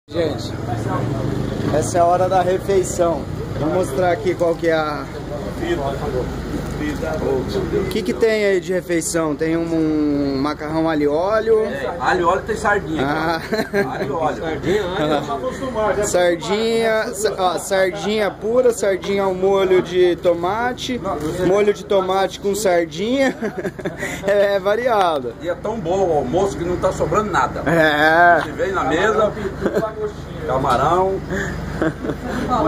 Carretera - Dia 3
La carretera de la muerte

Gustavo Narciso mostra o passo a passo de sua viagem pela Carretera de La Muerte. Desde a saída em São Paulo, de avião até Porto Velho, seguindo de moto para uma viagem que etá ao alcance de suas mãos.

A cobertura estará publicada na edição 137 da Revista Pró Moto.